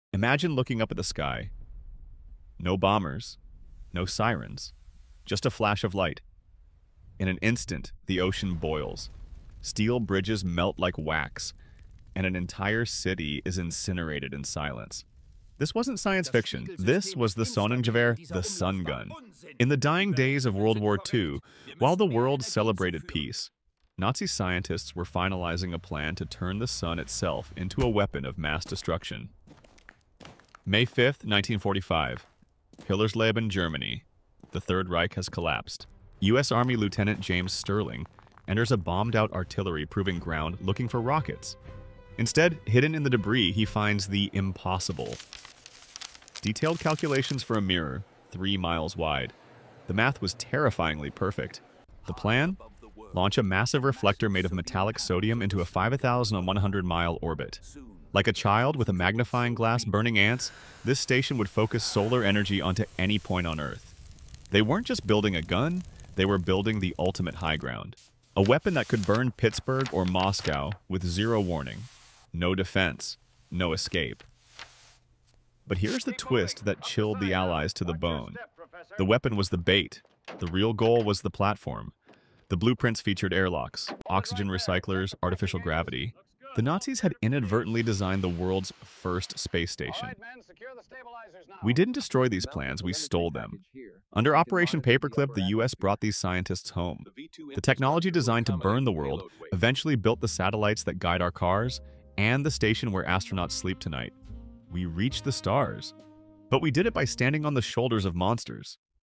Did Germany Try to Build a Death Star in 1945?
Did you know German scientists planned to build a giant mirror in space to burn cities? 🌍🔥 In 1945, US troops found the blueprints for the "Sonnengewehr" (Sun Gun). It sounds like sci-fi, but it was the grandfather of the modern Space Station.

From the secret files of Hillersleben to Operation Paperclip, discover the weapon that started the Space Race.

Some visuals in this video are AI-generated recreations, used solely to illustrate historical contexts and do not represent actual archival footage.